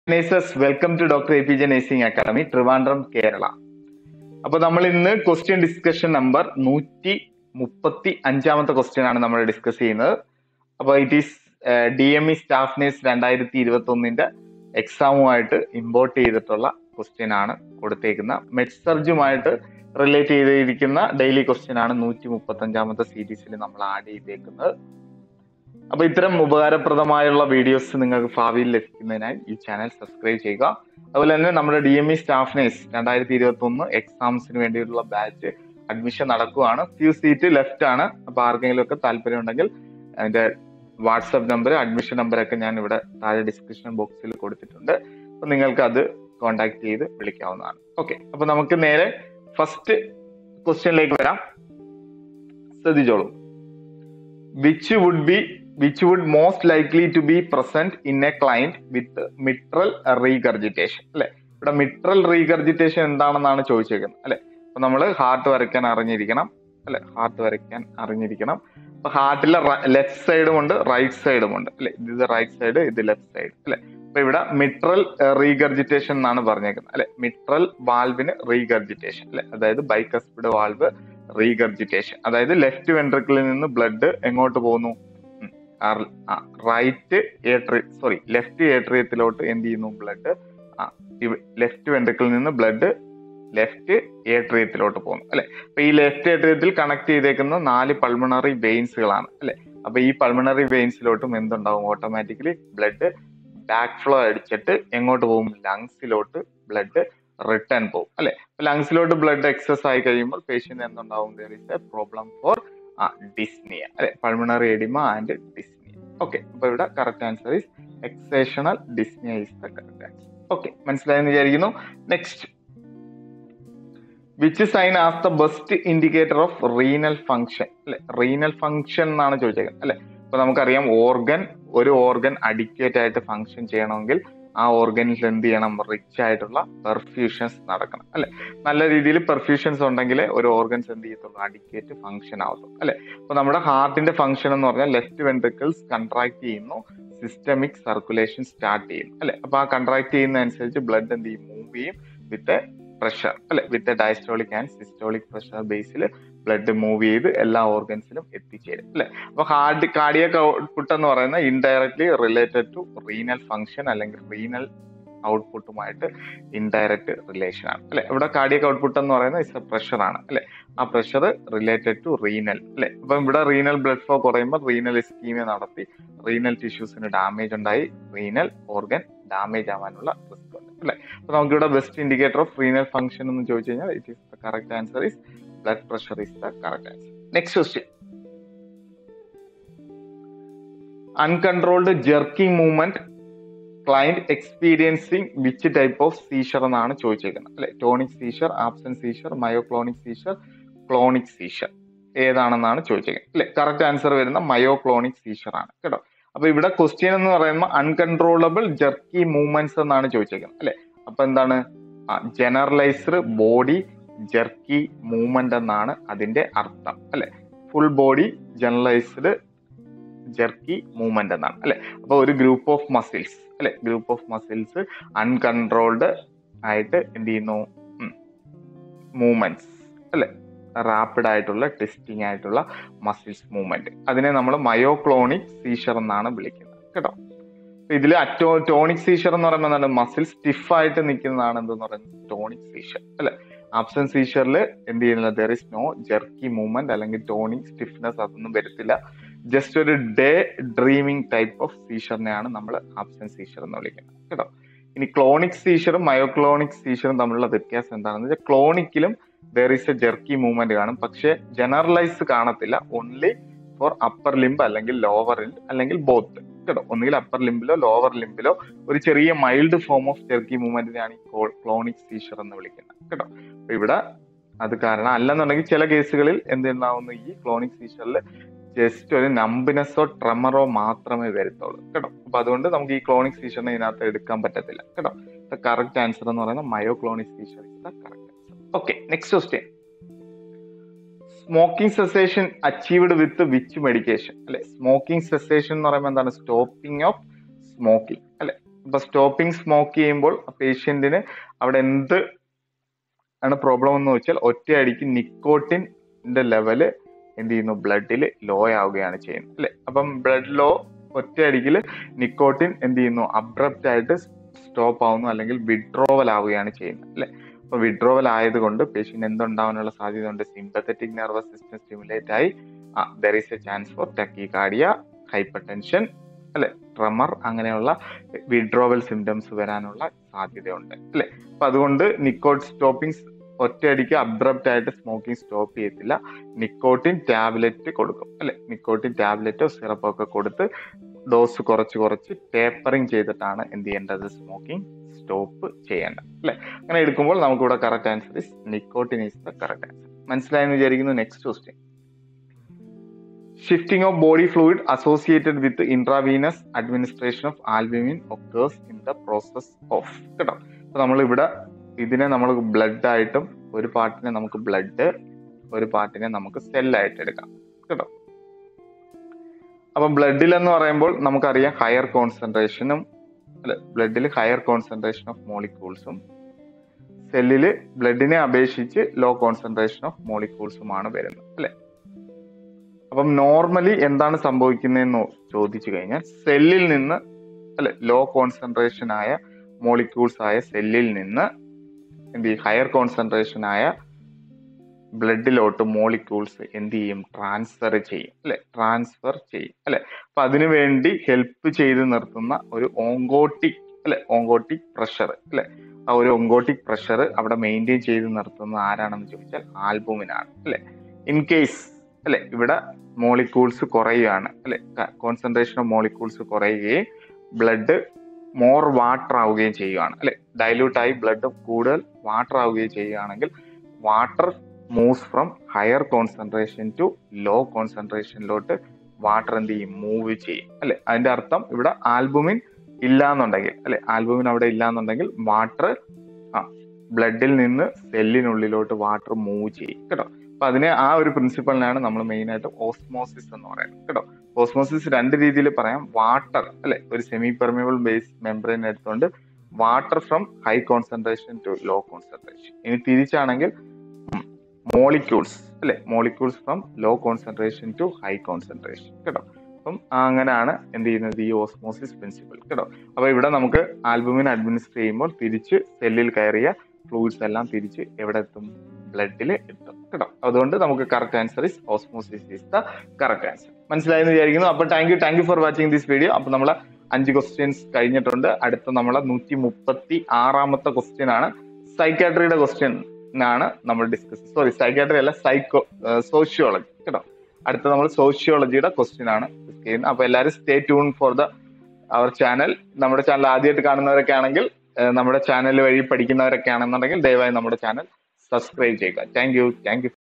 DME STAFF Nurse-2021 Exam Daily question discussion # 135
Hello Everyone ,
Good Morning Have a Nice Day

💐💐💐 Important Thought of the Day “”*All our dreams can come true, if we have the courage*””

Today’s Important Information
Daily Questions #135
*Answer Key  (rational explained in Dr APJ YouTube Channel)*
1. B. Exertional Dyspnea
2. A. Blood pressure
3. C. Myoclonic seizure
4. D. Nicotine (Nicotrol)
5. A. Osmosis

*Daily Questions #136 image attached*(sociology)

Best Regards 👍

India’s Leading
Dr APJ Nursing Academy
Trivandrum Kerala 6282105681 /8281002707 /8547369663